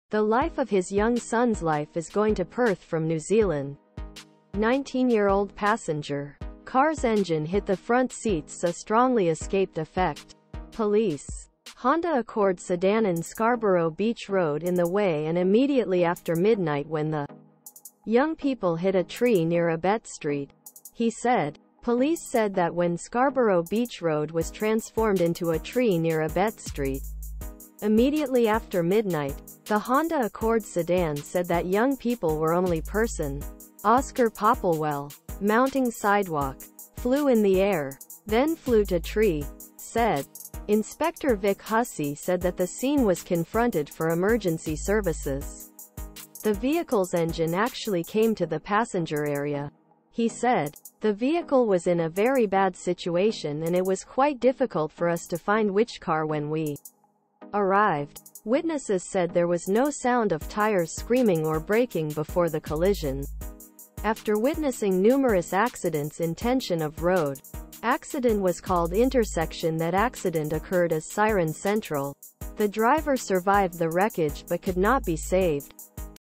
Scarborough crash: Mother travelling from NZ to Perth after son dies in horrific crash
The life of his young son's life is going to Perth from New Zealand. 19 -year -old passenger, car's engine hit the front seats so strongly escaped effect. Police, Honda Accord Sedan'ın Scarborough Beach Road in the way and immediately after midnight when the young people hit a tree near Abbett Street, he said. Police said that when Scarborough Beach Road was transformed into a tree near Abbett Street immediately after midnight, the Honda Accord Sedan said that young people were only person. Oscar Poppelwell, "Mounting sidewalk, flew in the air, then flew to tree."Said. Inspector Vic Hussey said that the scene was "confronted" for emergency services. "The vehicle's engine actually came to the passenger area," he said. "The vehicle was in a very bad situation and it was quite difficult for us to find which car when we arrived." Witnesses said there was no sound of tires screaming or braking before the collision. After witnessing numerous accidents in tension of road, accident was called intersection that accident occurred as "Siren Central". The driver survived the wreckage but could not be saved.